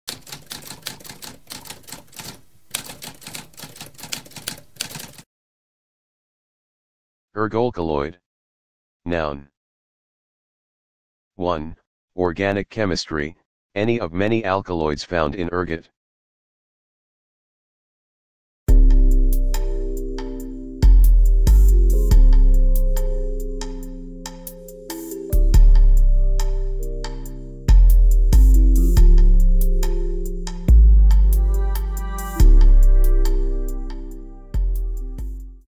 What does ergoalkaloid mean?
A spoken definition of ergoalkaloid.

Intro Sound:
Typewriter - Tamskp